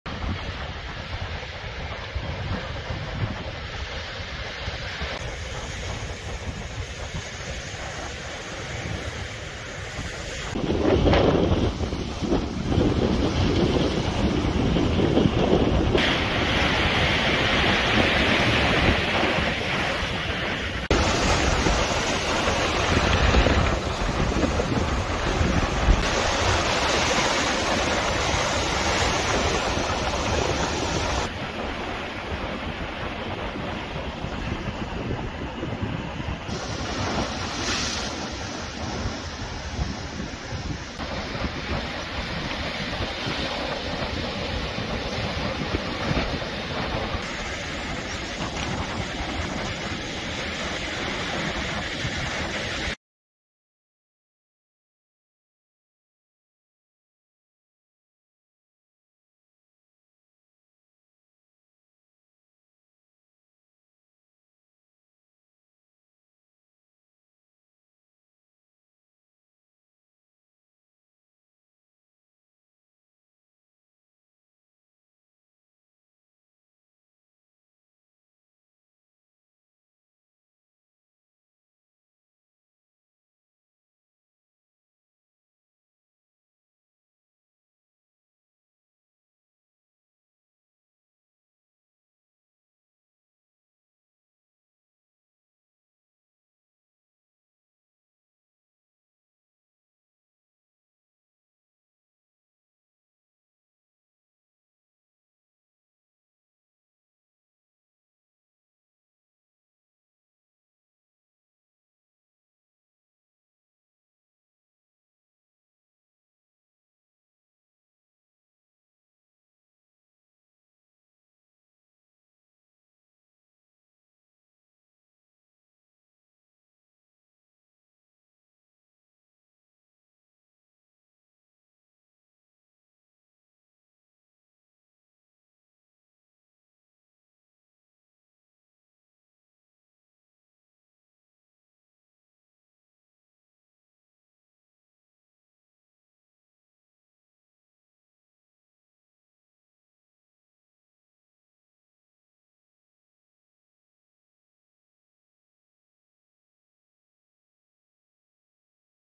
Marecchia in piena. La spiaggia di Rimini invasa dai detriti
Immagini del 17 maggio 2023 dopola piena del Marecchia.